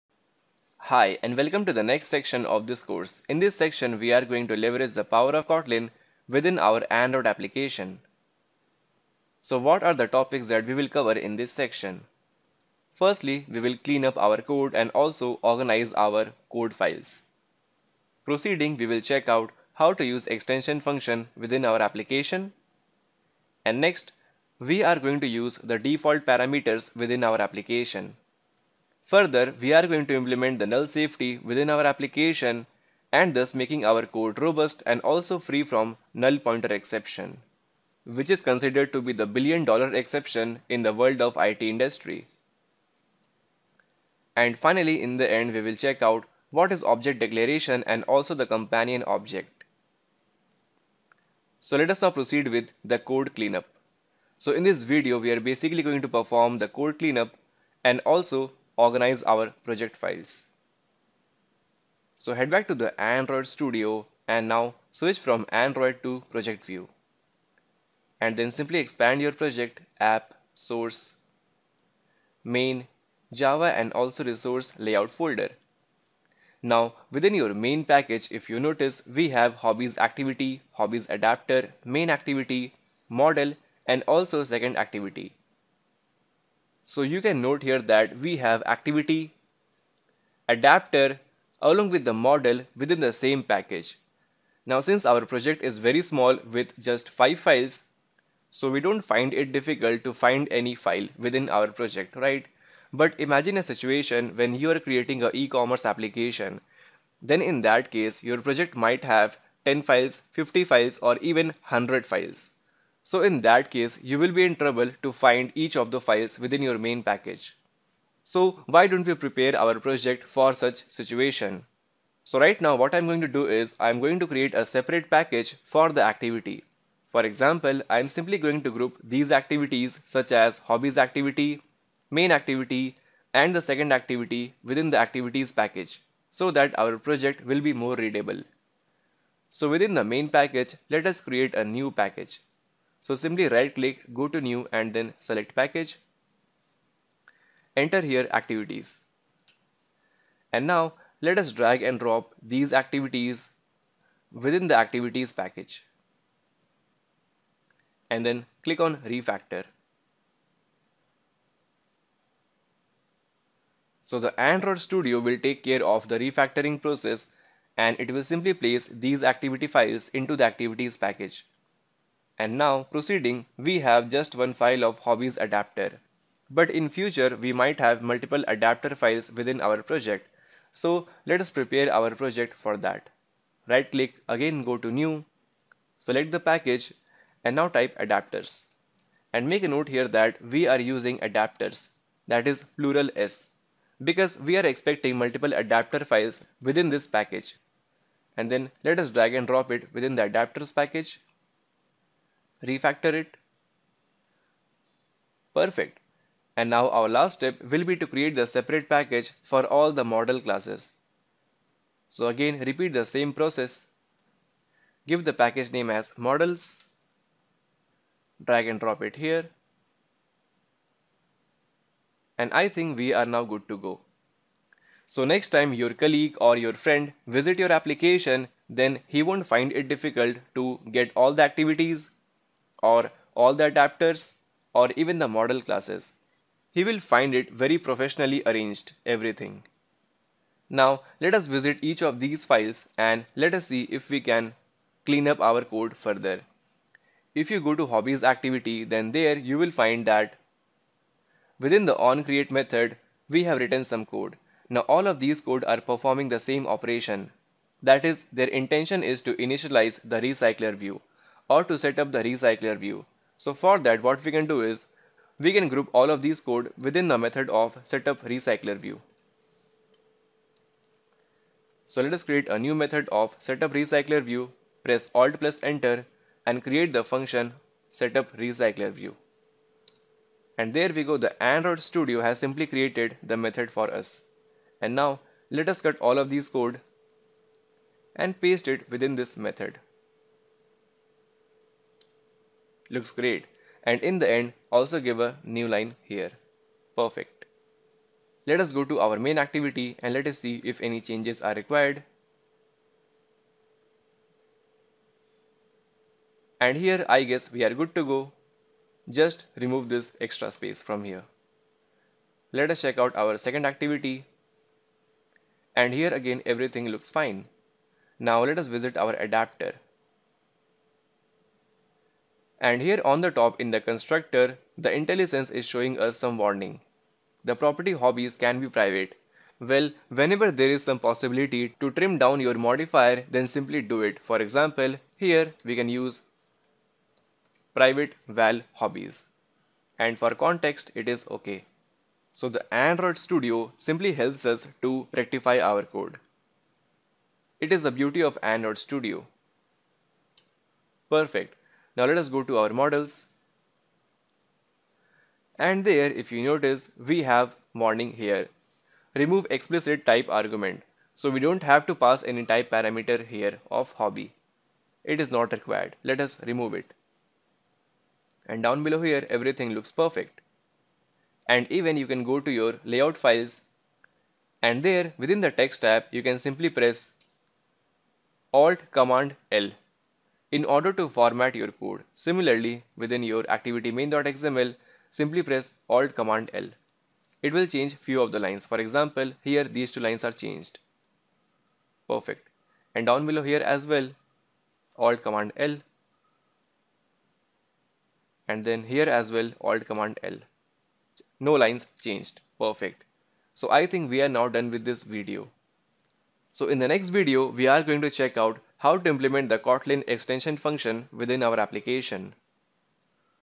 Android Kotlin Tutorial: Implement various Kotlin Features in Android App #4.1
Learn to use Extension Function, Default Parameters, Lambda Expressions, Object declarations, Companion Object, Null Safety operators such as Safe Call with let and Not Null Assertion Operator in Android App.